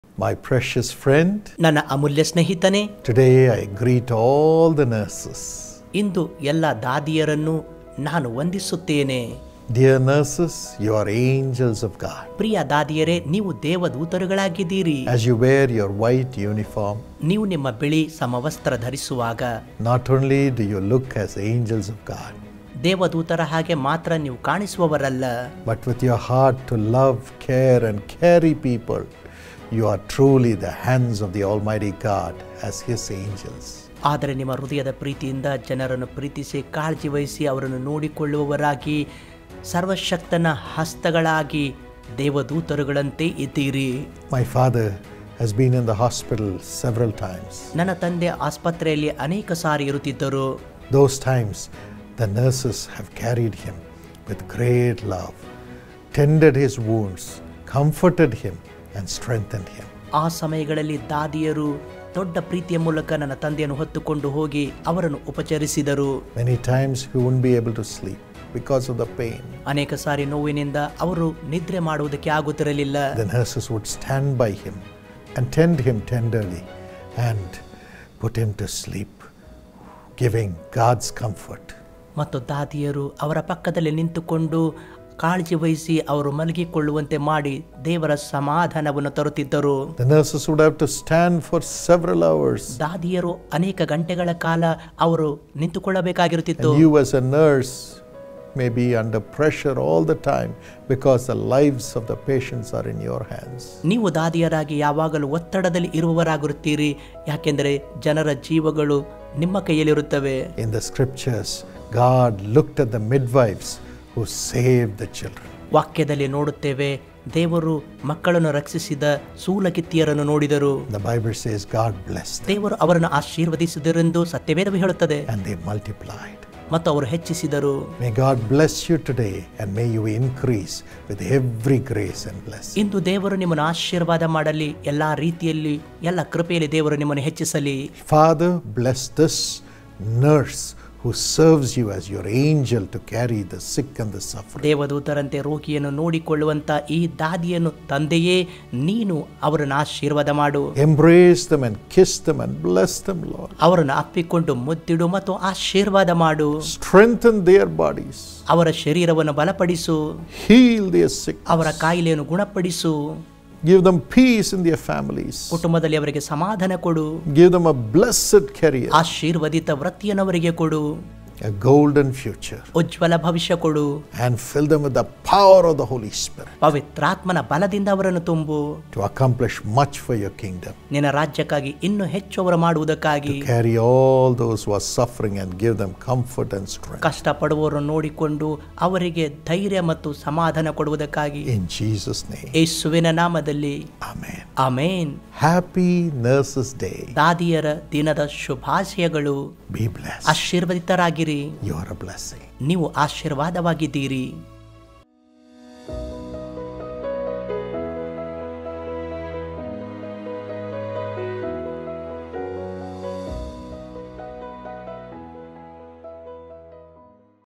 Angel of God - Nurses | Happy Nurses Day | Jesus Calls
In a world full of chaos and uncertainty, there are always those who bring light and hope. They are the angels of God, among us in the form of nurses. Nurses are the angels of God on earth, tirelessly working to heal and support their patients. They are truly the unsung heroes of our society, working tirelessly to make a difference in the lives of others. Let us honor and appreciate these healthcare heroes for their selfless work every day